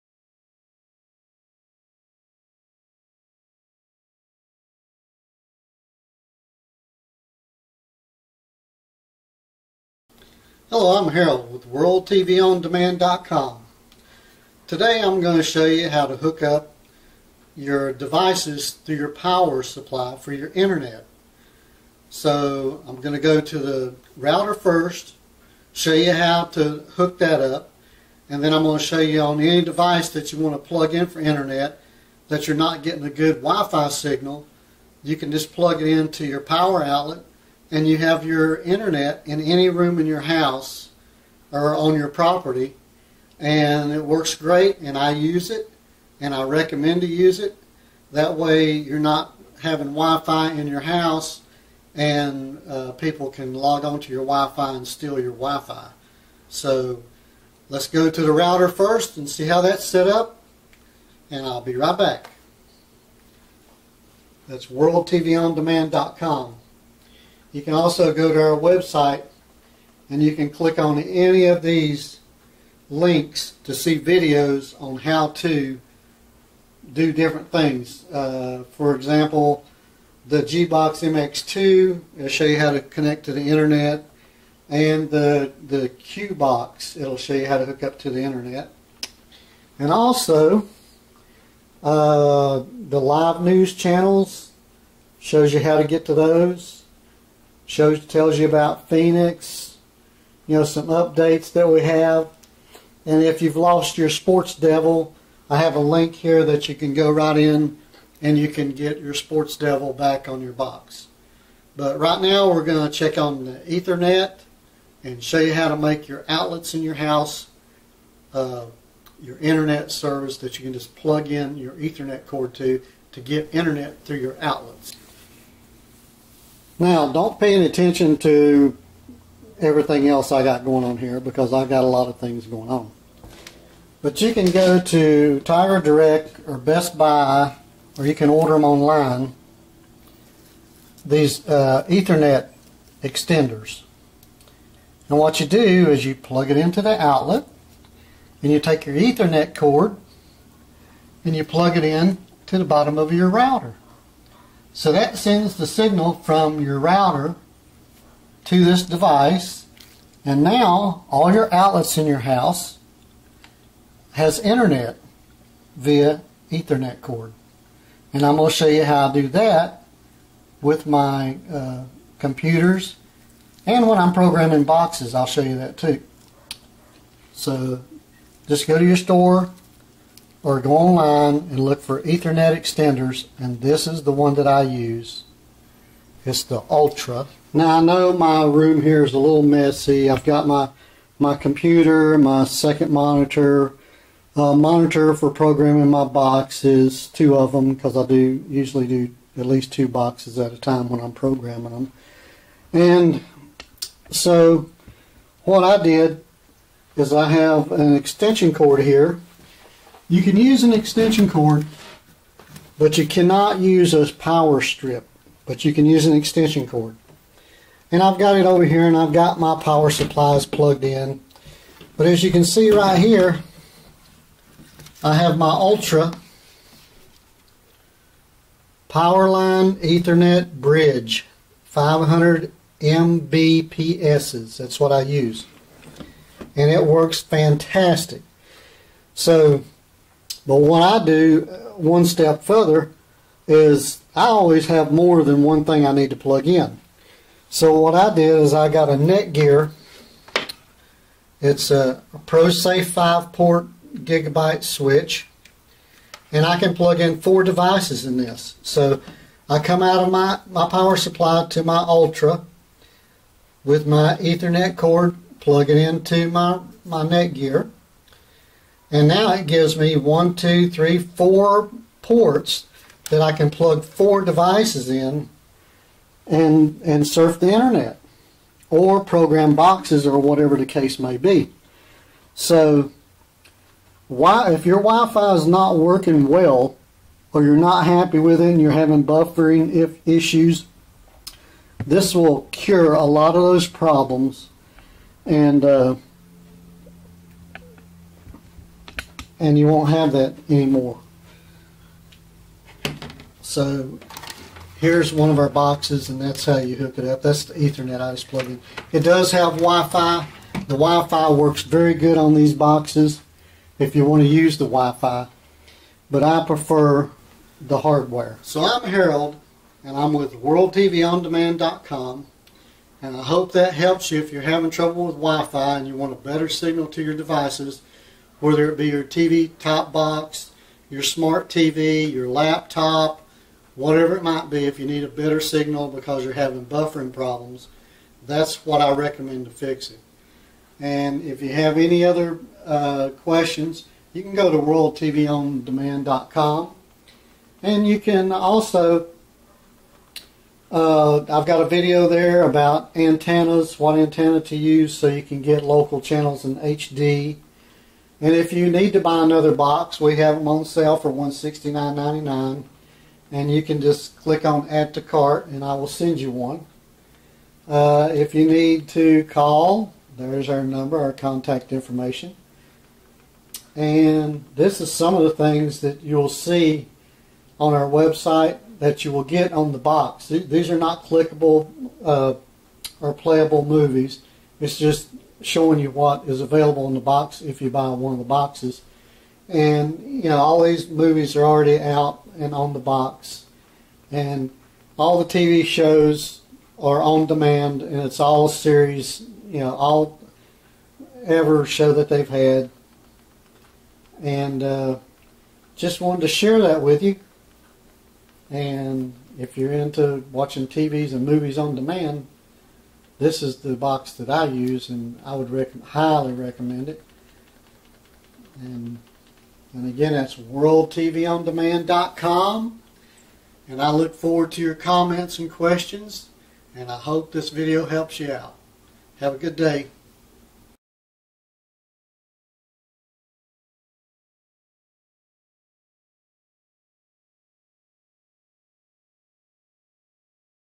Did you know you can get Internet through your power outlets?
You can get your internet through your power outlets. In this video I will show you how to connect to the internet through your power lines. You will need internet service and the Ultra Powerline Ethernet Bridge 500 Mbps device. You can connect as many devices as you need. They come two in a pack.